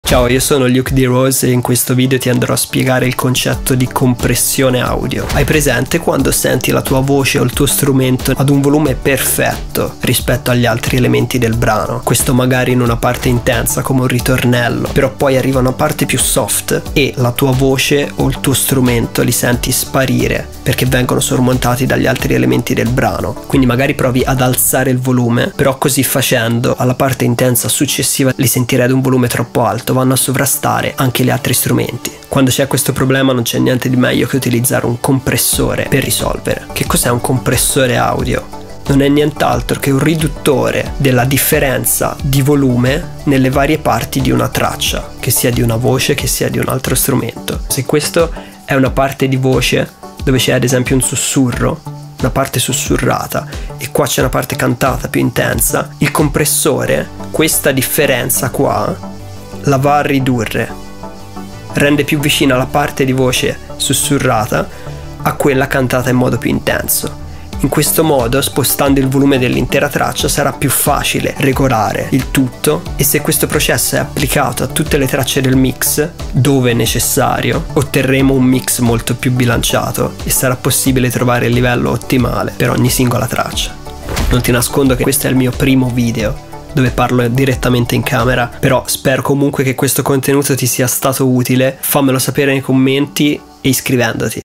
COMPRESSIONE AUDIO: Il Concetto Spiegato Semplice (In Meno Di 2 Minuti)
Spero che questo mio contenuto ti sia tornato utile nel comprendere il concetto di Compressione Audio, non nascondo di non essere perfettamente a mio agio nel parlare in camera perché questo è il mio primissimo video. Però mi impegnerò nel migliorare un video dopo l'altro.